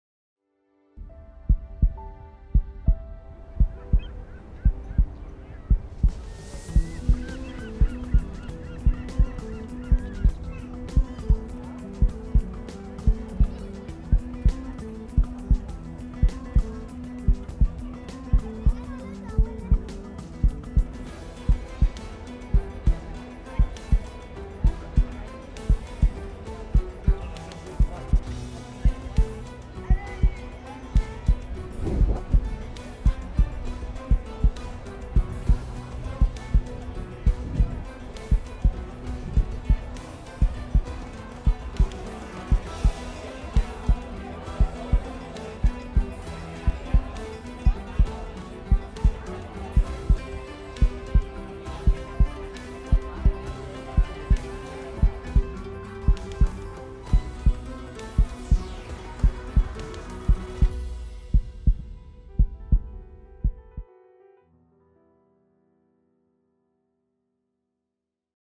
Donosti VR 360
Donostia, San Sebastian, Basque Country
This 360º video shows Donostia-San Sebastián, in all its glory.
Spain is alive in You.